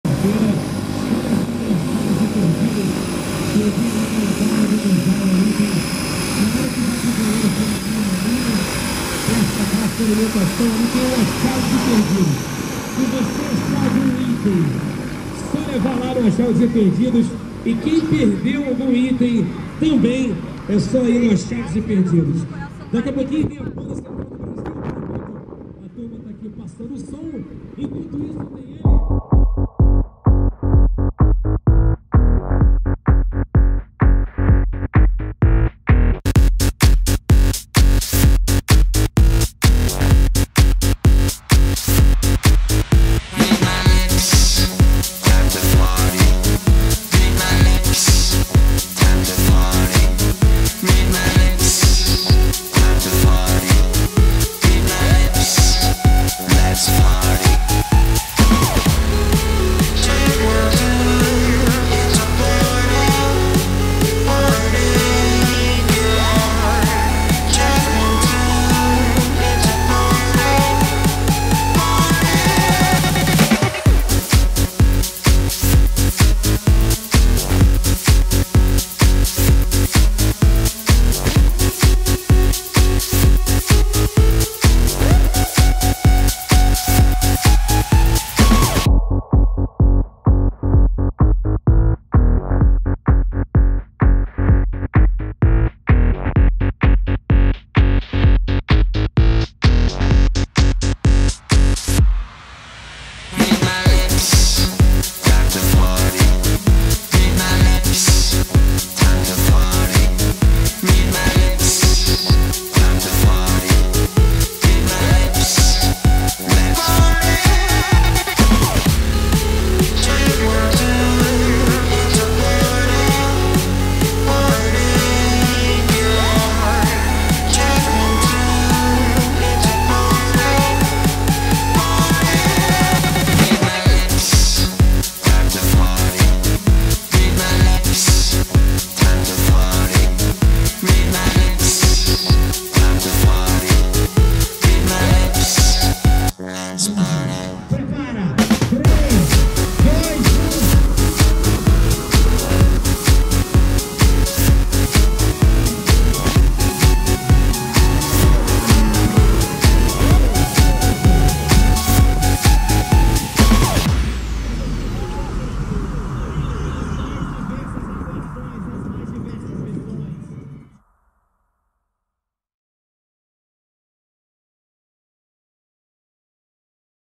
EP.11 Embraer C-95BM Bandeirante da Força Aérea Brasileira e equipe de Paraquedismo Os Falcões.
Mais um show muito bacana dos paraquedistas da equipe os falcões.